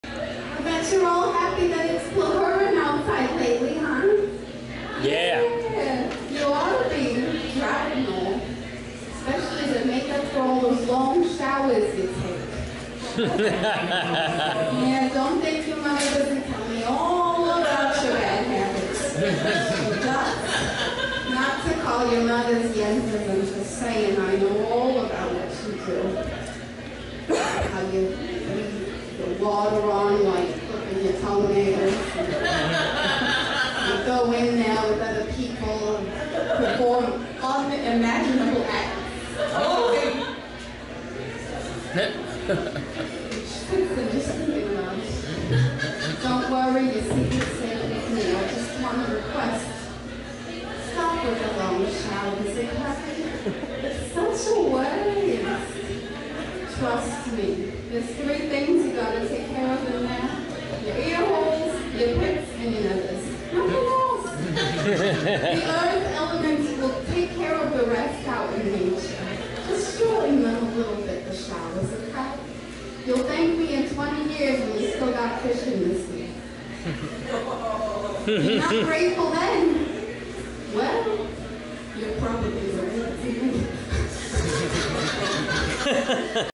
Regina Pickel
Regina lectures audience about the California drought.
@ Woods Bar & Brewery in Oakland, February 2016